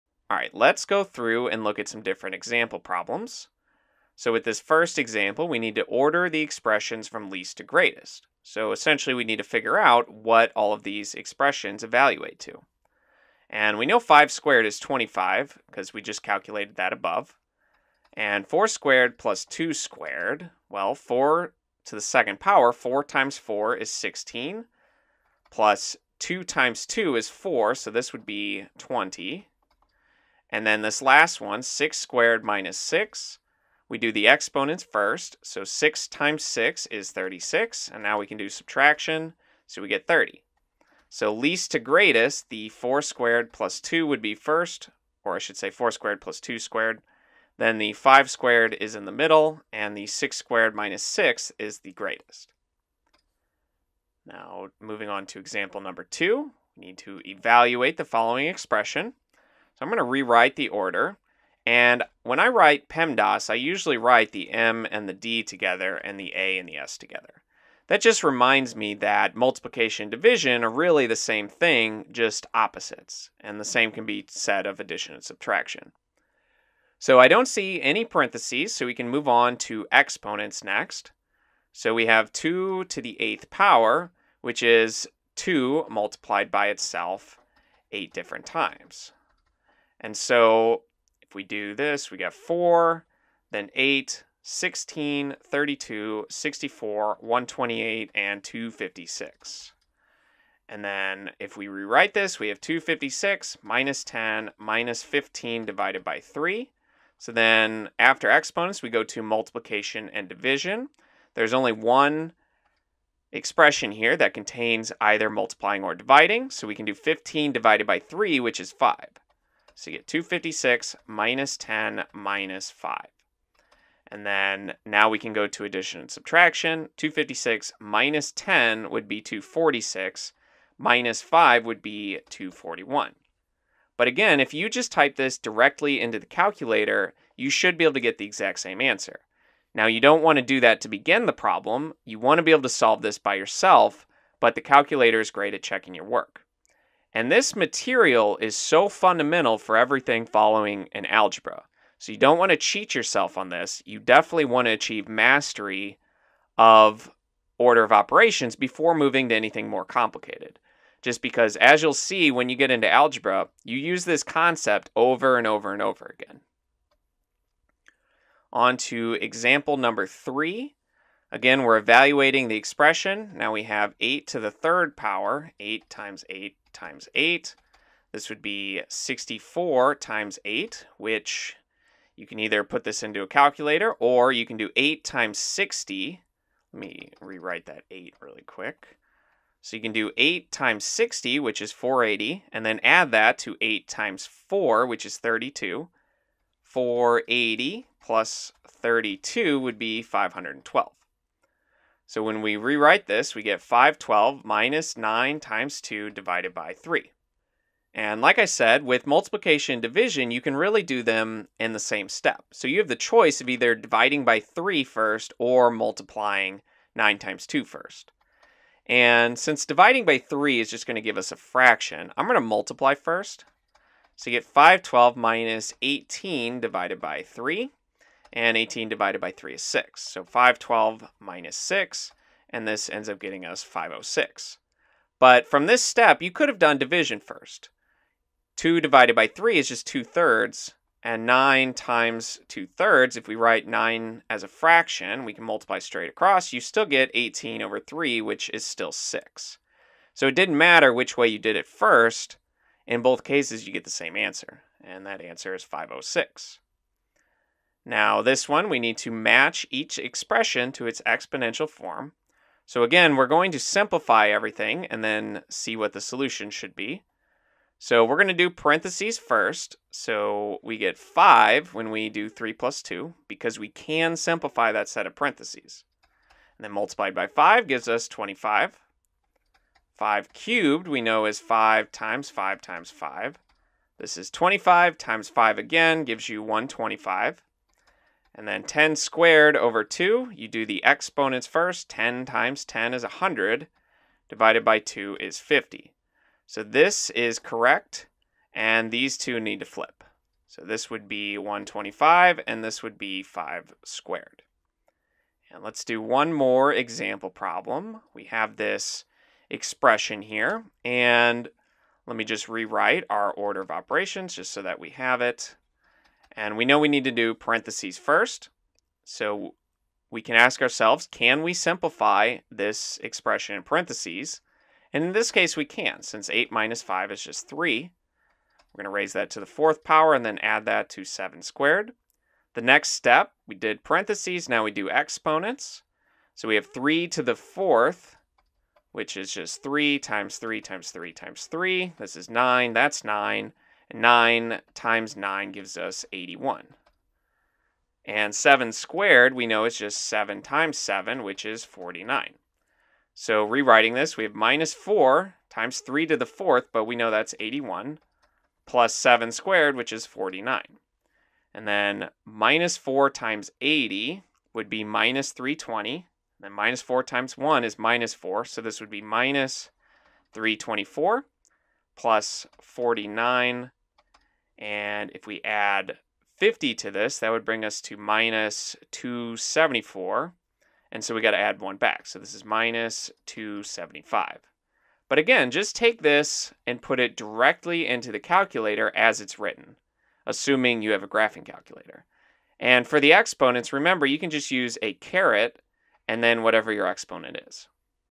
Order Of Operations: Examples
This video introduces the order of operations while showing several different example problems. Essentially, the order of operations shows us which way we should evaluate different expressions, starting with the parenthesis, then moving onto exponents, multiplication/division, and lastly to addition and subtraction.

2.

EulersAcademy.org